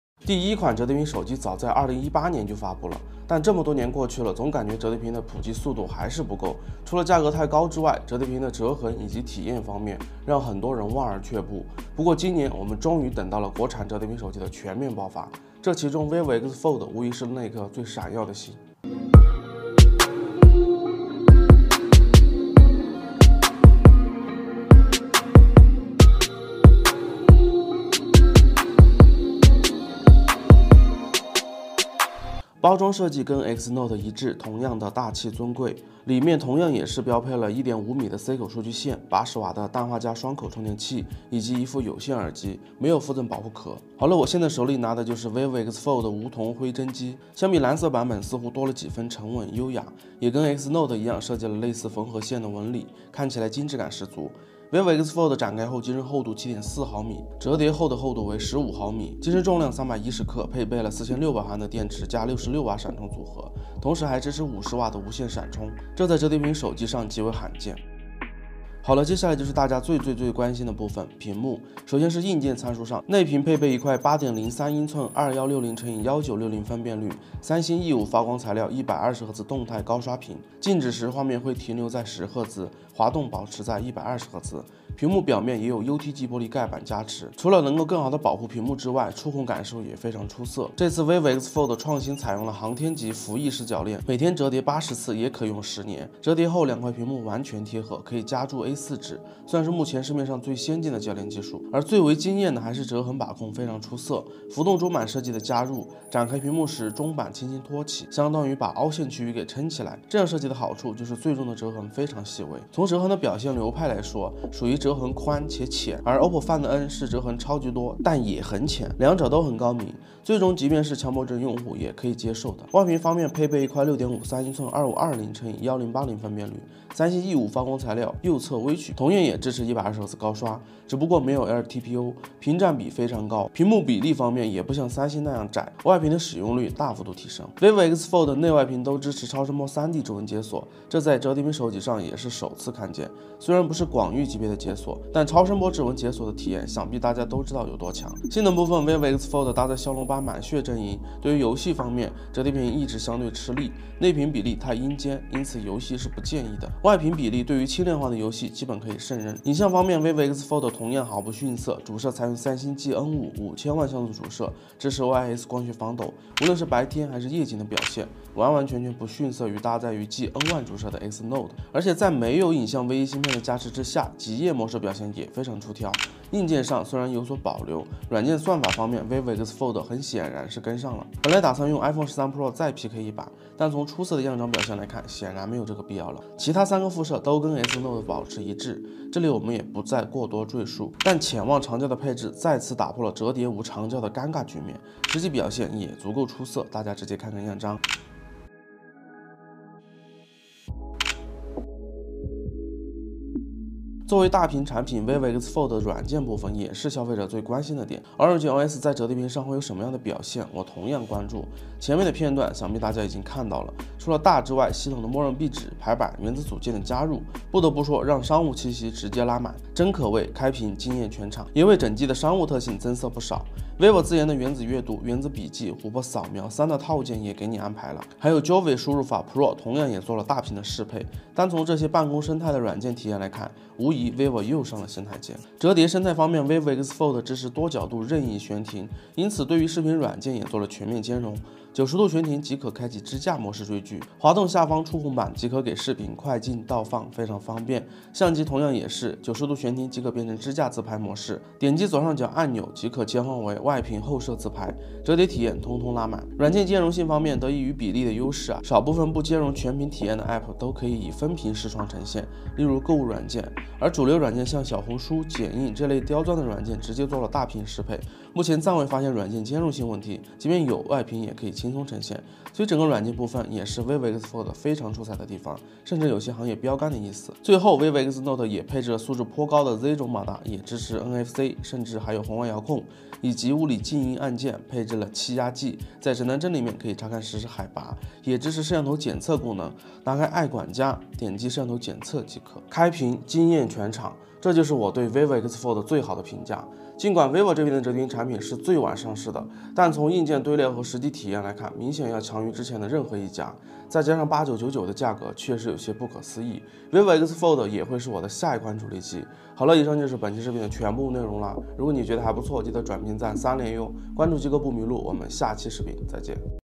Vivo X Fold Unboxing and Hands On Review-Gaming & Camera Test BestTech
Vivo X Fold Unboxing and Hands On Review BestTech